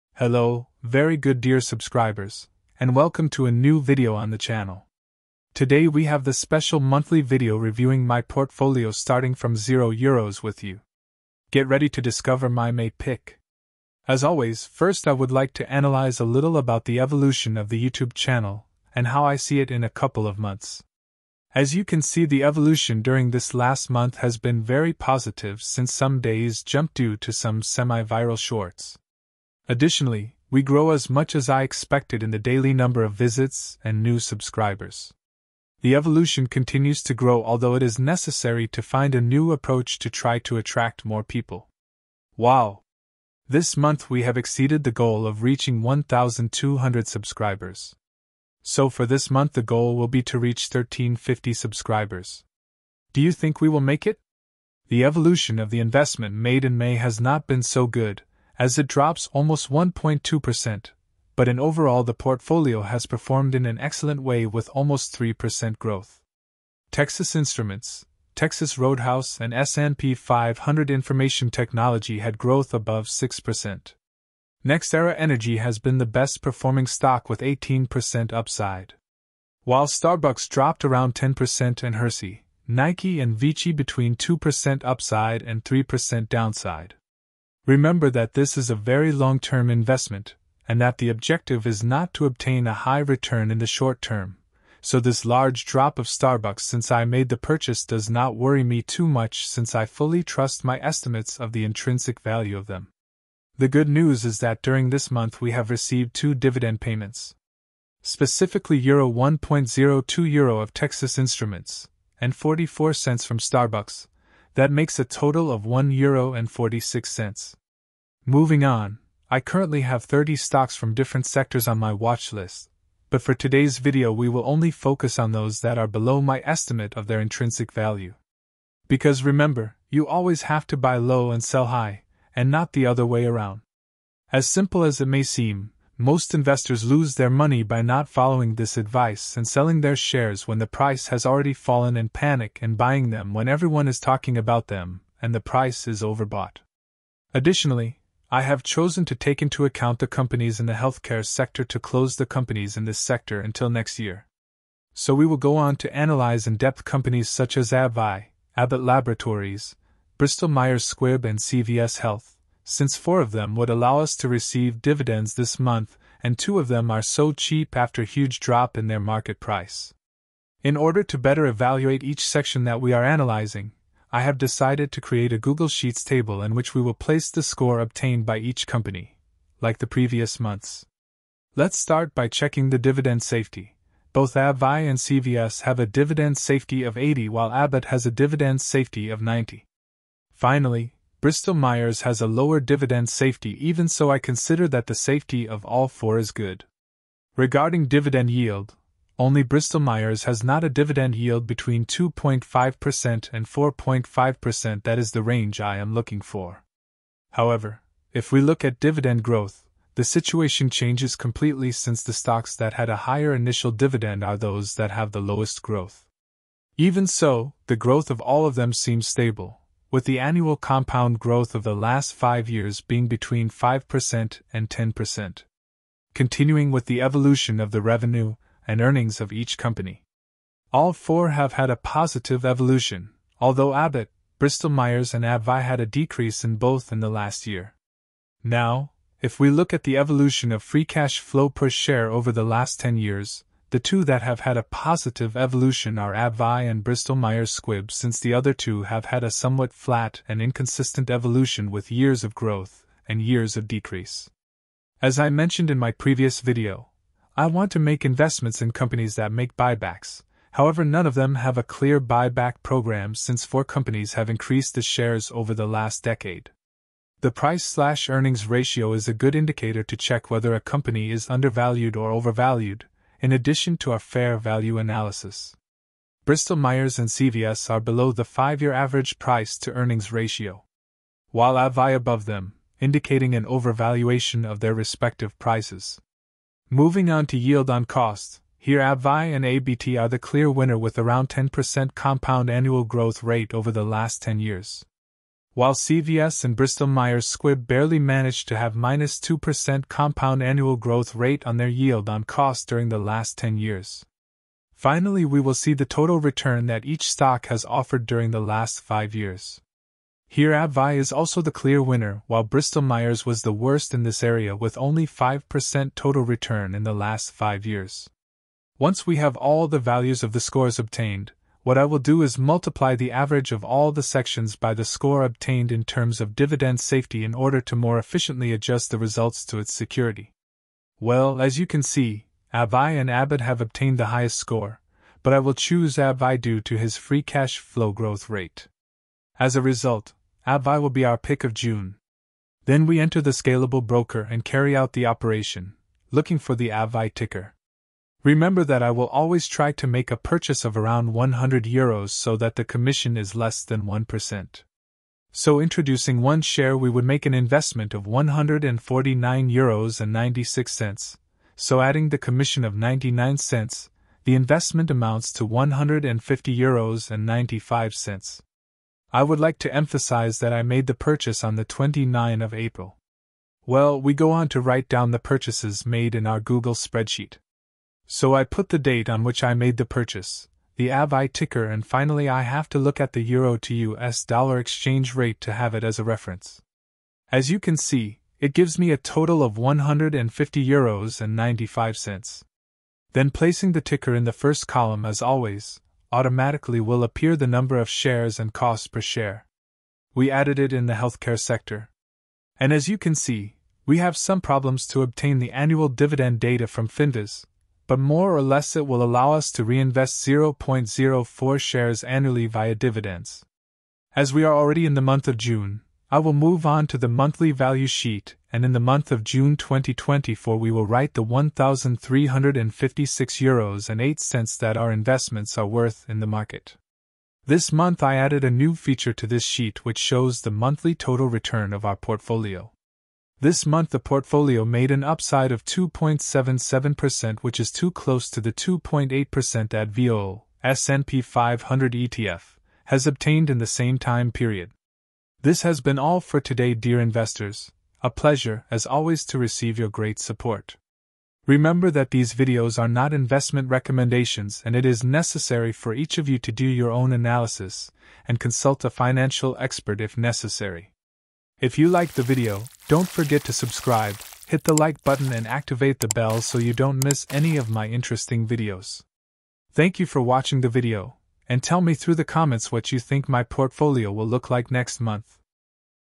What happens this month with the Stock Market? #10
Join me in this month's deep dive into my investment portfolio, starting from €0! 🚀 Discover the highs, lows, and my strategic moves for June. 📊
We've chosen scalable as our broker, offering low fees and a powerful platform ➡️ Let's make informed investments together!
With just €100 monthly and 1% commissions, we're targeting long-term success.
💹 Investment Insights:
Analyzing May's performance, dividends received, and a steadfast long-term perspective. 📉📈
📋 Watchlist Exploration:
Dive into my watchlist of 30 stocks from diverse sectors, focusing on those below intrinsic value. Learn the art of buying low and selling high. 💰
🔍 Company Spotlight:
In-depth analysis of Bristol Myers, Abbott Laboratories, AbbVie and CVS Health. Uncover dividend safety, yield, growth, and financial metrics. 📈💹
🚀 Investment Decision:
Abbvie emerges as the June pick after a drop in the last month! Follow my step-by-step process on Scalable Broker and witness the strategic purchase. ☕💸
📈 Google Sheets Analysis:
Check out the organized Google Sheets table, evaluating each company's score for a comprehensive view. 📊 🚨@rynewilliams GOOGLE SPREADSHEET🚨 tracks investments, sector allocations, and future portfolio values
🔔 Monthly Update & Future Outlook:
Track the monthly value, new investments, and predictions for the upcoming month. Stay ahead of the game! 📅
🤓 Financial Literacy:
Emphasizing the importance of individual analysis and consulting financial experts. Your financial journey, your responsibility. 💡💼
Join the community and let's navigate the markets together! 🌐🔔 Remember, these videos are not investment recommendations. 💬💼